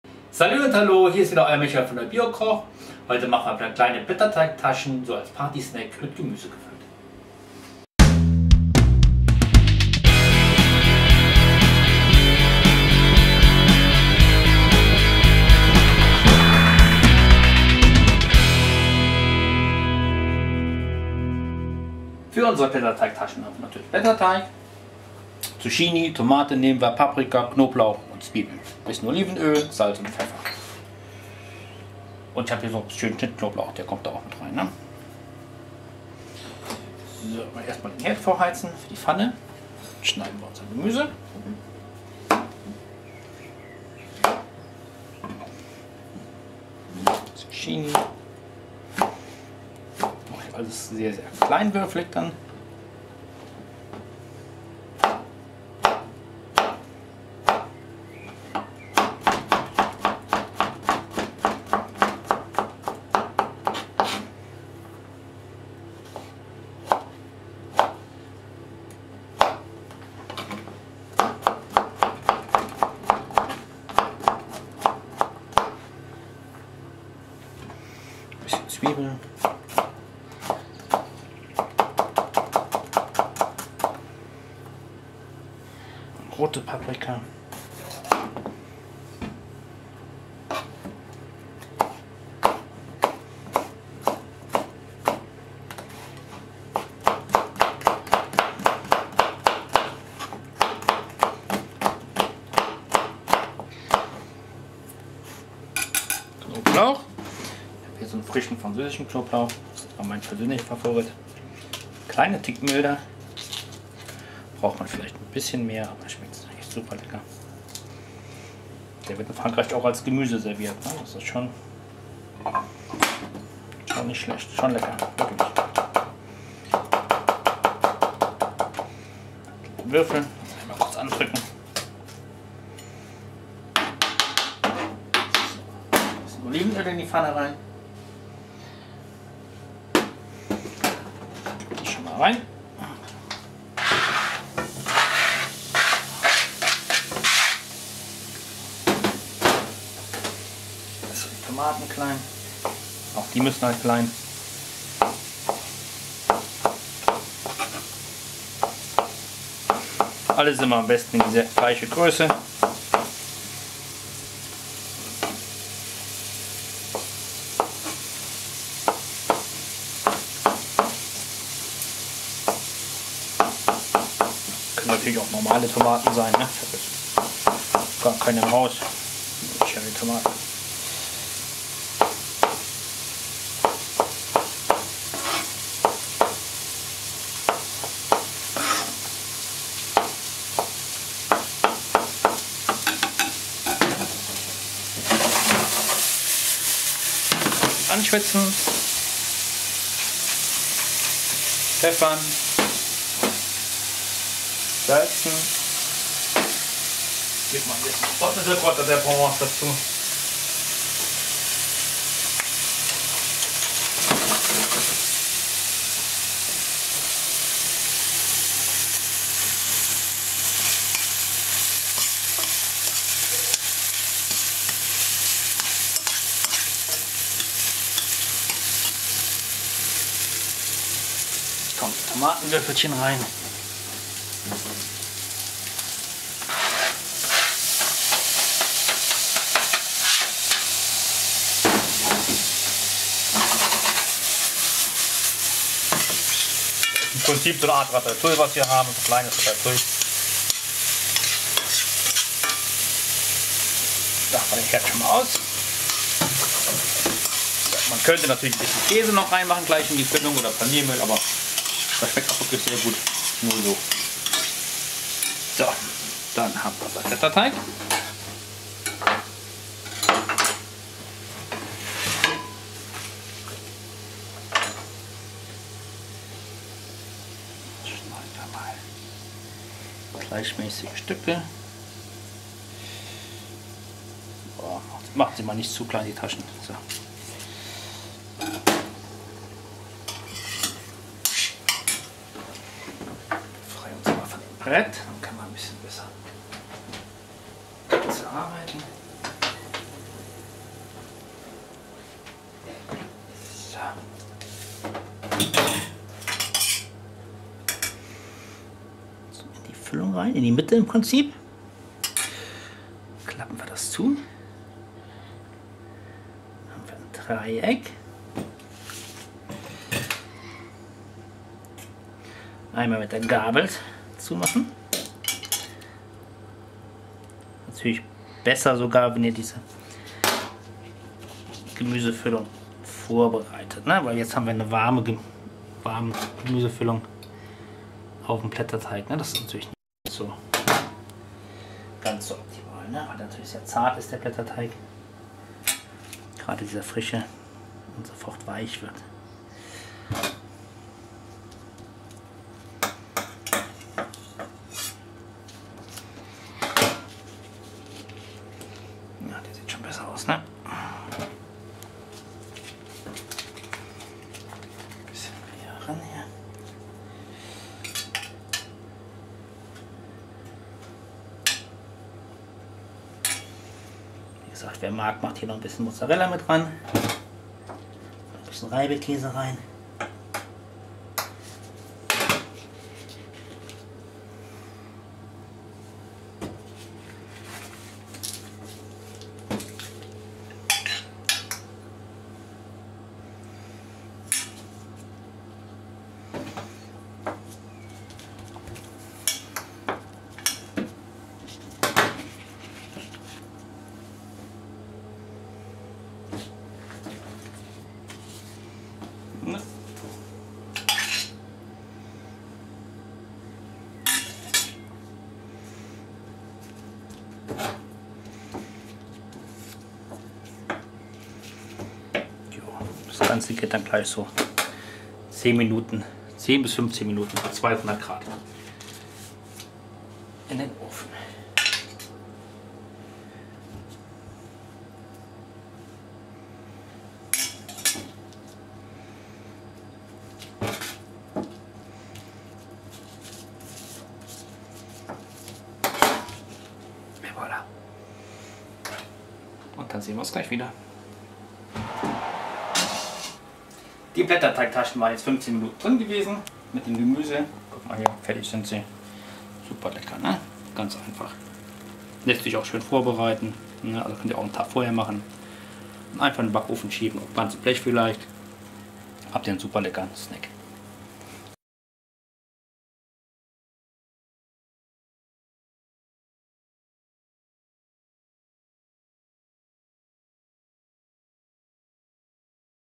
Blätterteigtasche Rezept vegetarisch vegan -- Der Bio Koch #225
So macht man Blätterteigtaschen vegetarisch vegan.

Rezept pro Person
• 200g Blätterteig
• ¼ Paprika rot
• ½ kleine Zucchini
• 1 Tomate oder 3 Cherrytomaten
• Knoblauch
• ½ Zwiebel
• Olivenöl
• Pfeffer
• Salz